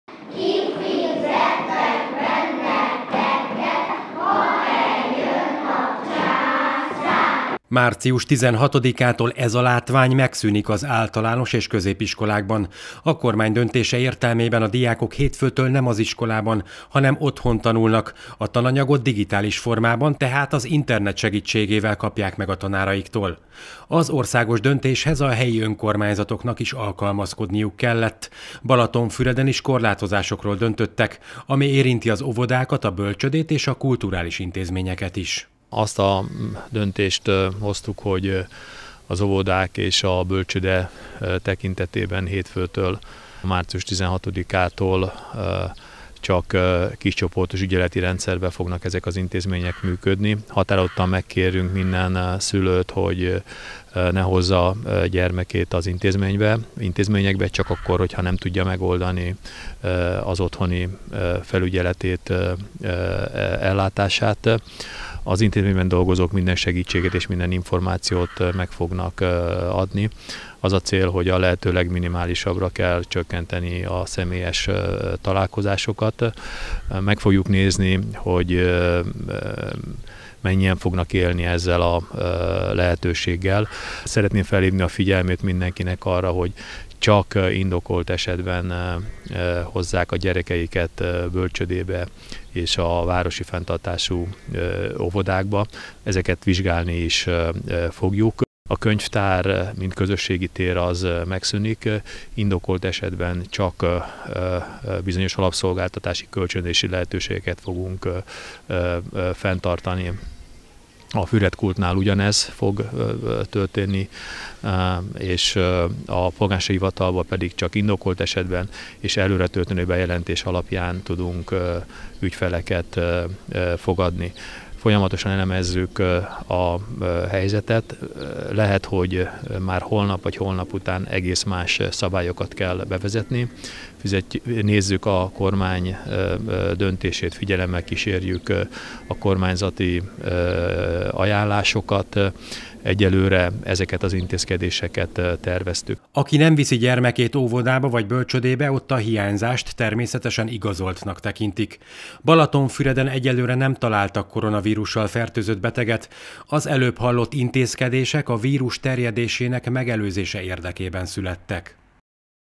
Dr. Bóka István polgármester a helyben bevezetett korlátozó intézkedésekről nyilatkozott
A koronavírus veszélye, a kór terjedésének megakadályozása érdekében számos intézkedésről döntött a balatonfüredi önkormányzat. Március 16-tól több kulturális intézmény bezár, illetve részlegesen működik, a családoktól pedig azt kérik, aki teheti nem vigye gyermekét óvodába vagy bölcsődébe, mert ezek az intézmények is csak ügyeleti rendszerben fognak működni. A város vezetése azt javasolja, mindenki kerülje a zsúfolt közösségi tereket, különösen vigyázzunk az idősekre.